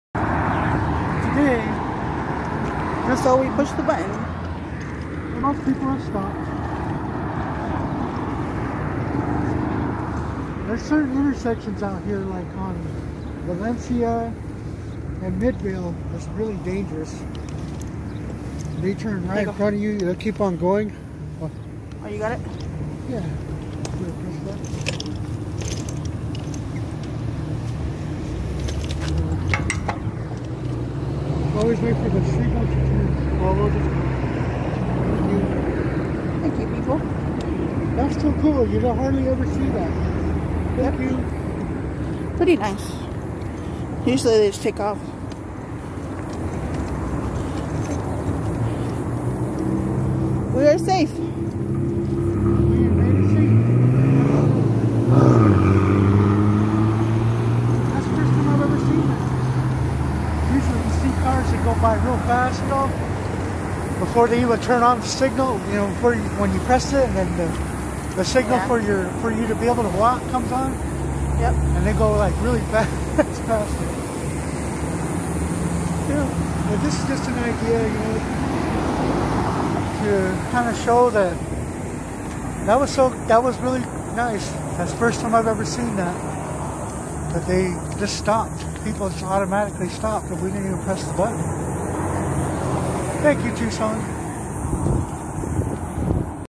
A day in the life of a Roadrunner
Crossing the street was easier than other days here in tucson, AZ